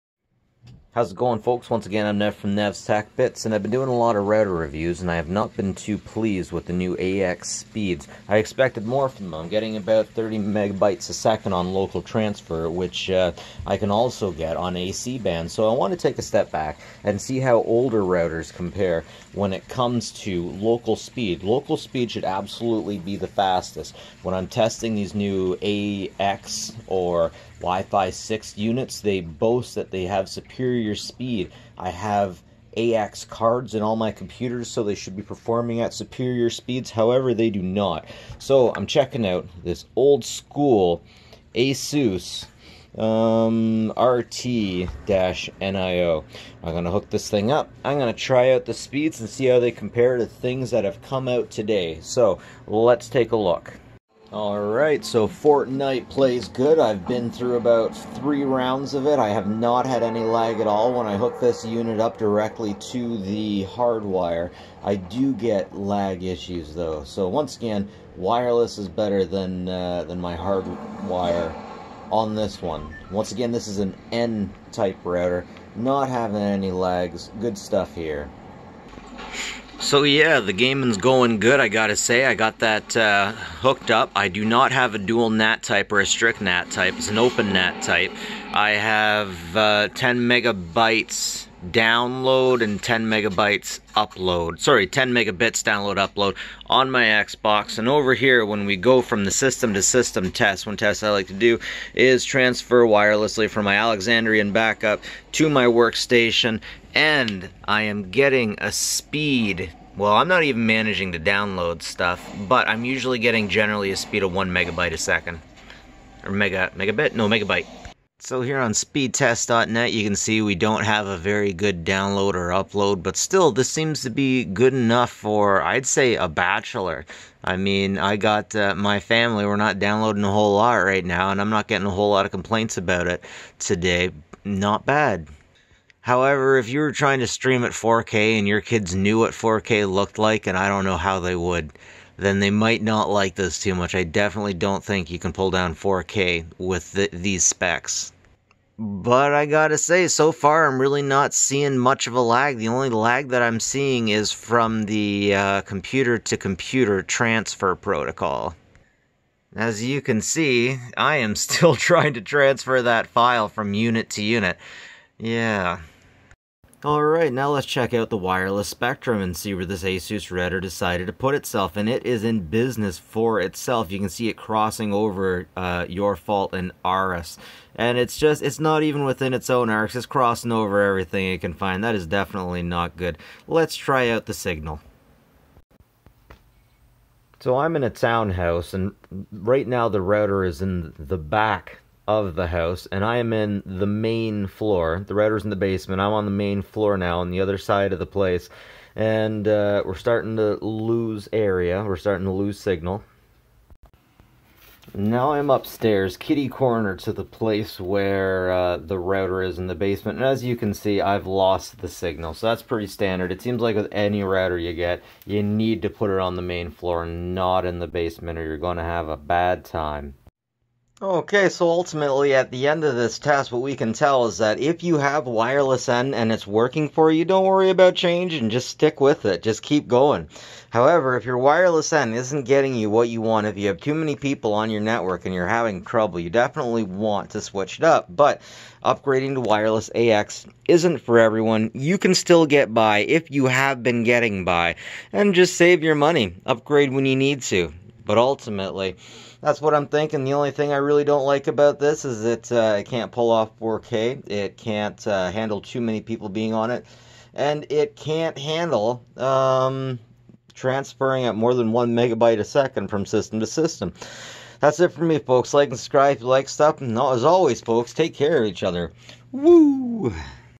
Is Wifi 4 (802.11n) "N" Still Any Good In 2020? Benchmark, Analysis, Review Asus RT-N10+
Wifi 4 or N was released in 2009. A lot has gone on since this time.

802.11n (Wi-Fi 4)
The first standard to specify MIMO, 802.11n was approved in October 2009 and  allows for usage in two frequencies - 2.4GHz and 5GHz, with speeds up to 600Mbps. When you hear wireless LAN vendors use the term “dual-band”, it refers to being able to deliver data across these two frequencies.